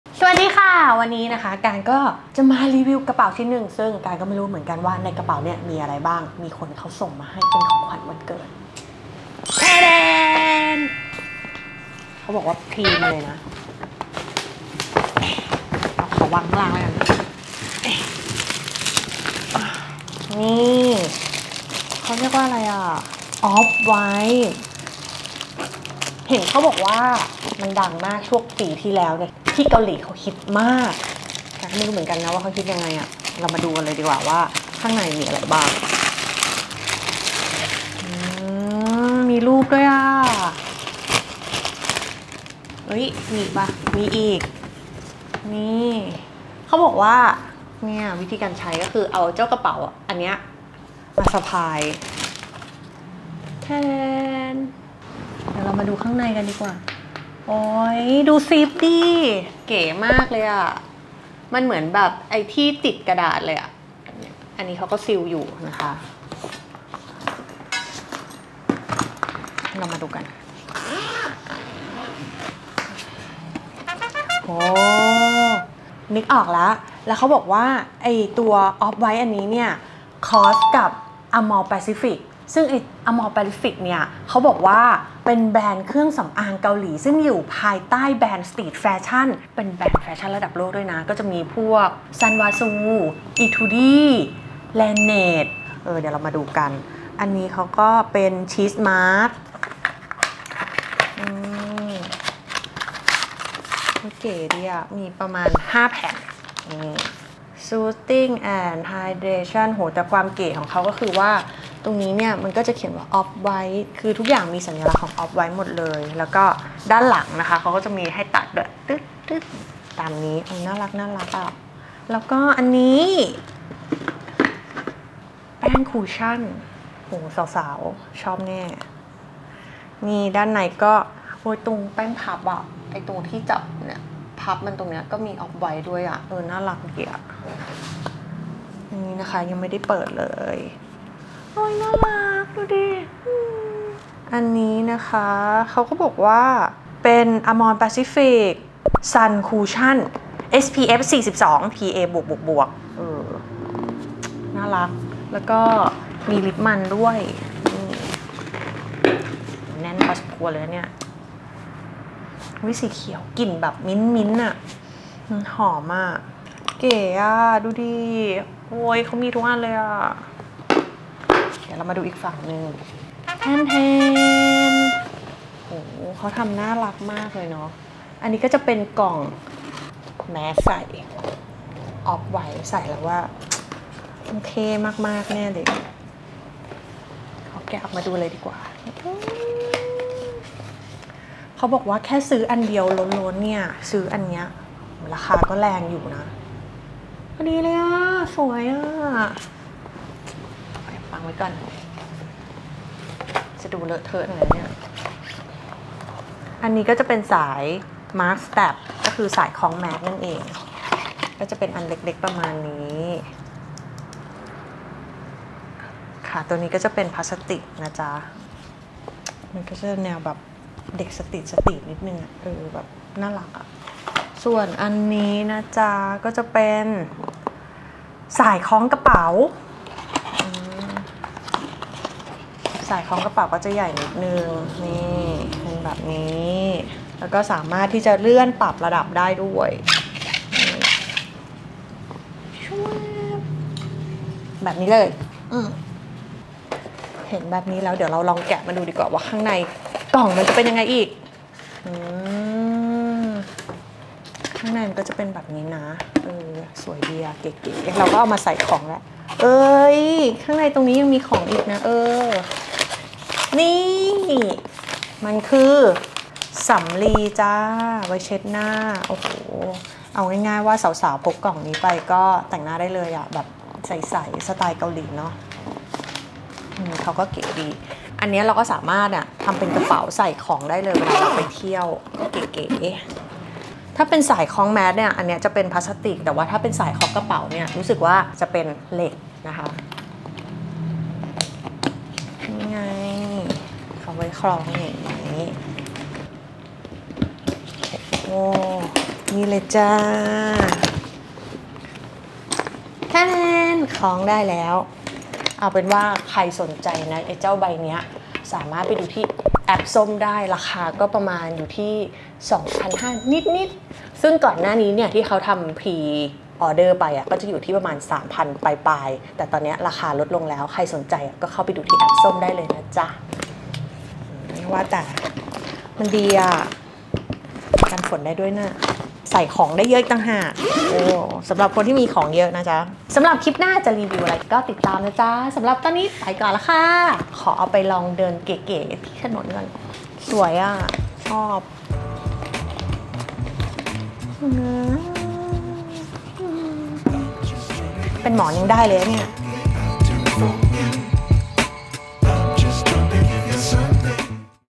UNBOXING PROTECTION BOX OFF WHITE x AMOREPACIFIC | กานต์ OK
เปิดกล่องแห่งความลับ “PROTECTION BOX” จาก Off-White™ x AMOREPACIFIC มาดูกันว่าข้างในมีอะไรบ้าง?